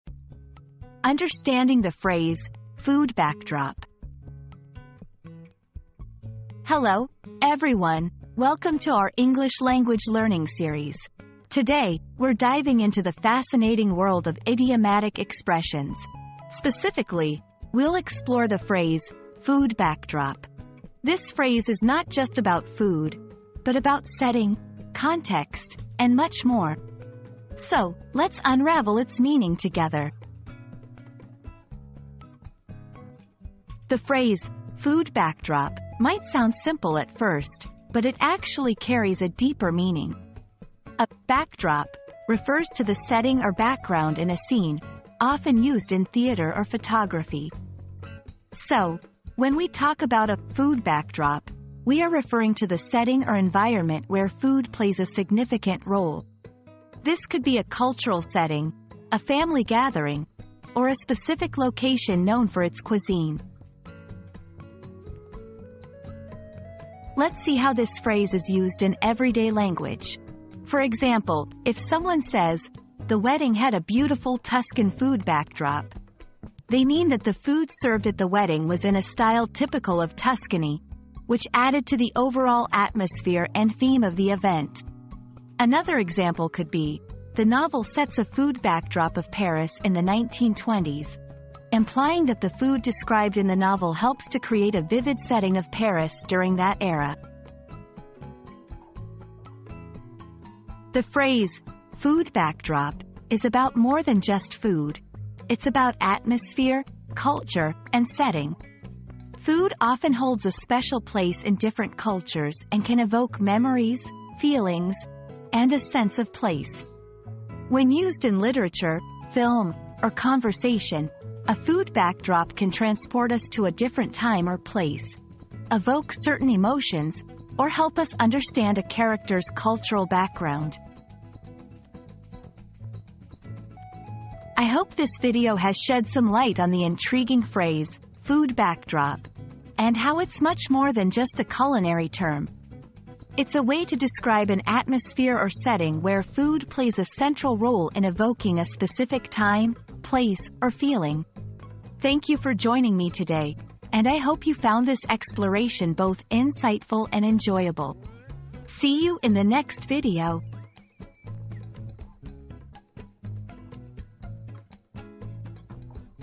Understanding the Phrase "Food Backdrop"
Unraveling the Mystery of the Food Backdrop • Discover the true meaning and significance behind the intriguing phrase 'Food Backdrop' in this captivating video. Gain a deeper understanding of how this term is used and its relevance in various contexts. Join us on this journey of unraveling the mystery behind the Food Backdrop phenomenon.

00:00 • Introduction - Understanding the Phrase "Food Backdrop"
00:33 • What Does "Food Backdrop" Mean?
01:06 • Examples in Everyday Language
01:40 • Why It's More Than Just Food